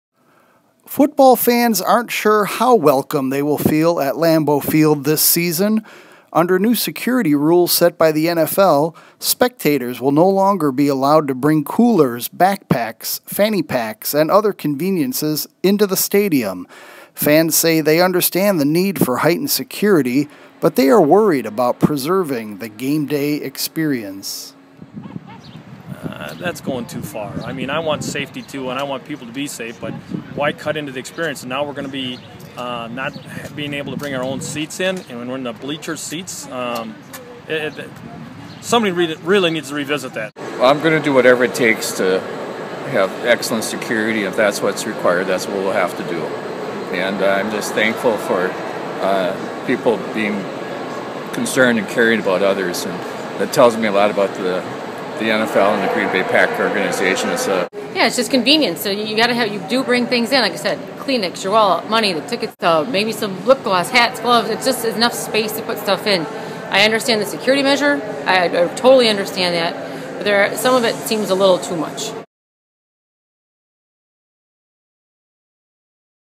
Packers fans sound off on new stadium restrictions
Thursday, June 13: Green Bay Packers fans are bracing for new stadium security rules that prohibit such game-day accessories as fanny packs and seat cushions.